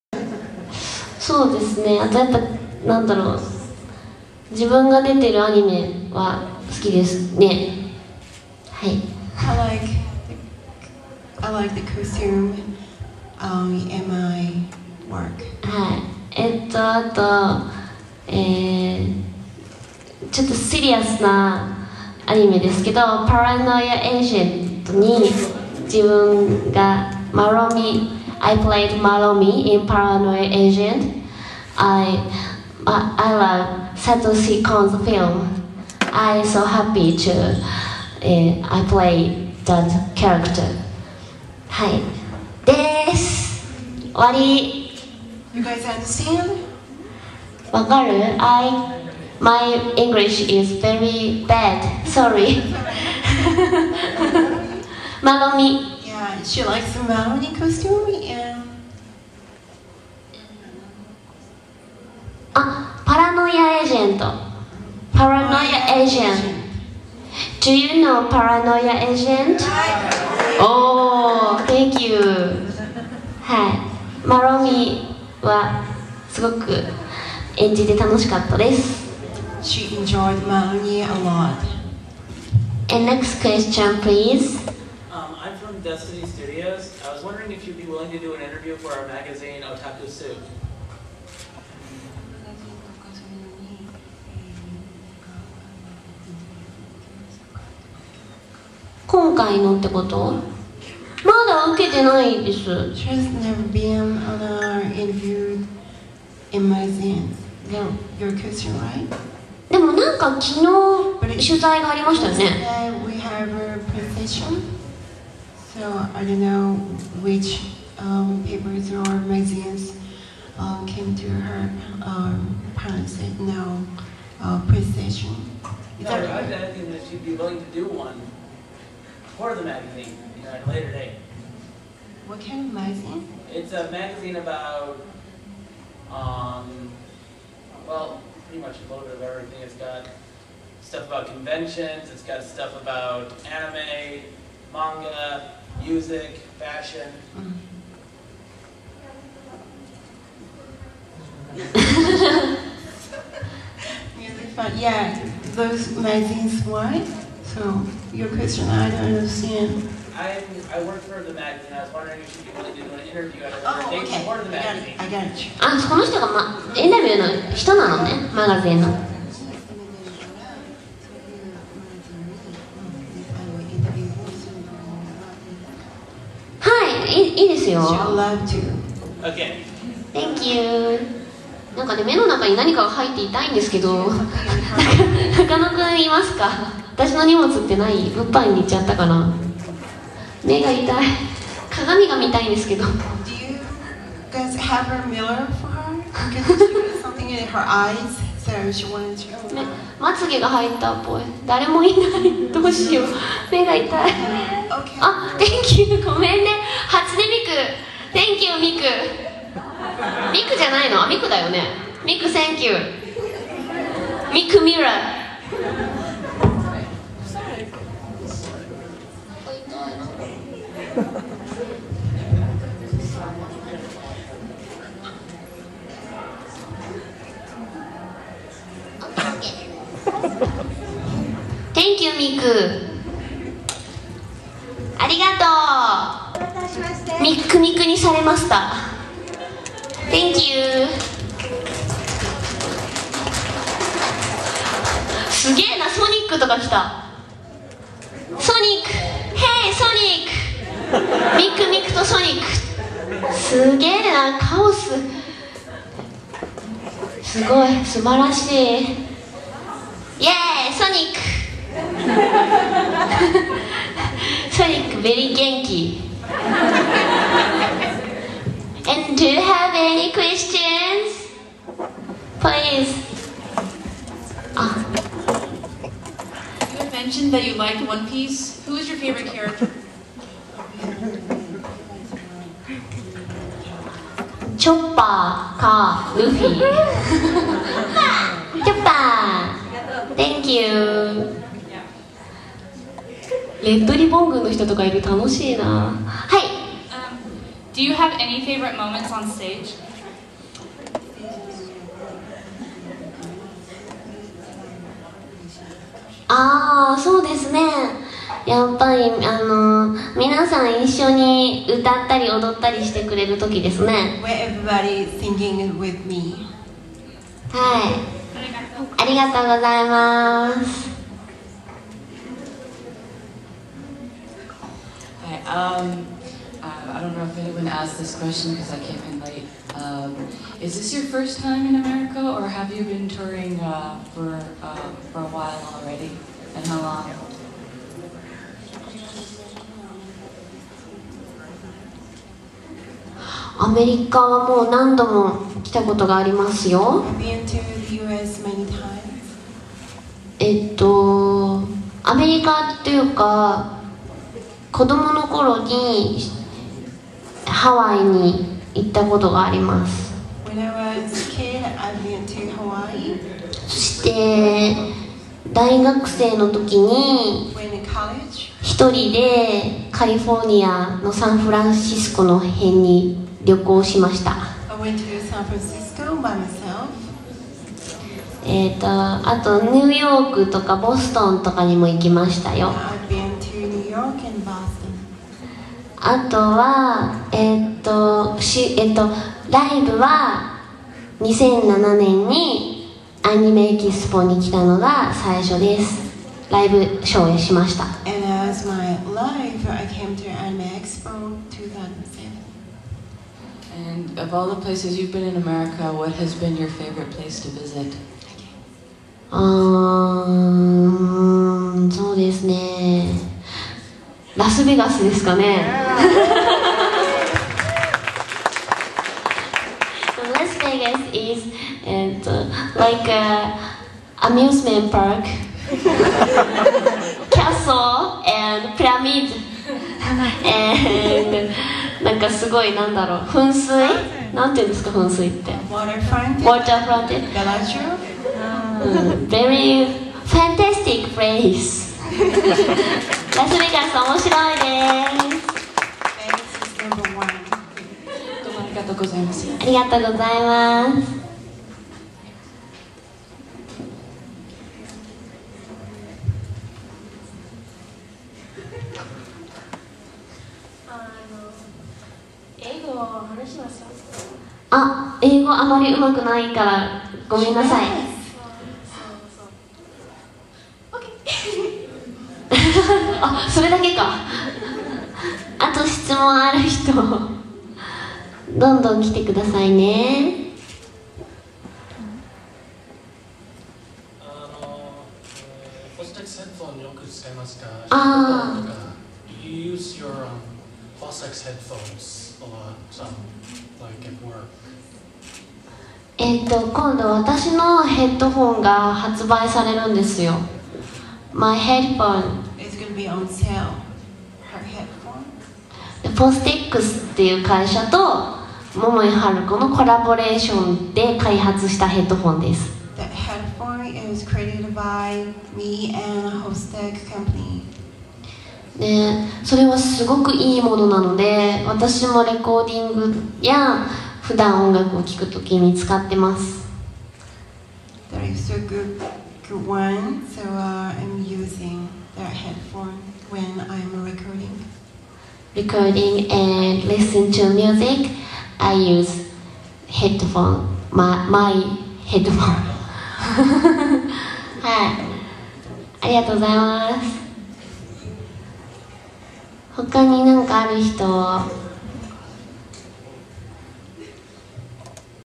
Anime Vegas 2009 - Momoi in the Morning Part 2
This panel took place on Sunday morning at Anime Vegas 2009.  It was not in the original schedules and was added that weekend.  This panel features a Q&A session with Halko Momoi.  This is part 2.